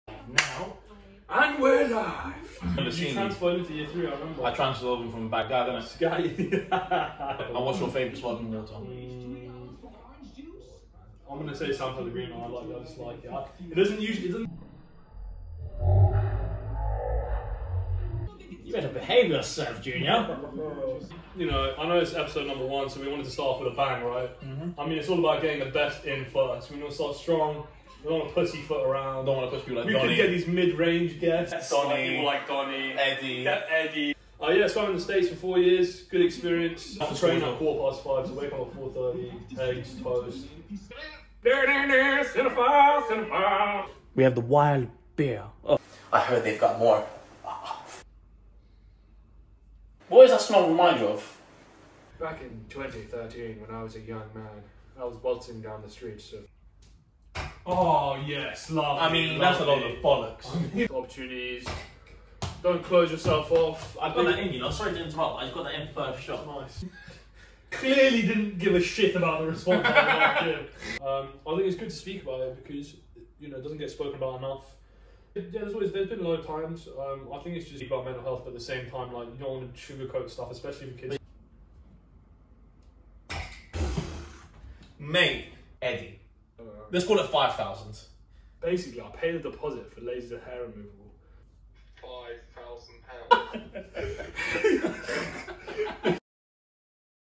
W-Talk TRAILER
Follow @W_TALK_ on Instagram!

Here’s the trailer for episode 1 of W Talk.

Episodes will be released every Sunday at 6pm GMT.

Episode 1 with @OLLYKEEGAN1 will be released on the 9th August 2020.

20 years of friendship + a free box of Lagers & IPA’s from @BEER52HQ through @REVOLUTAPP, contributed to real conversations, challenges, pranks and more!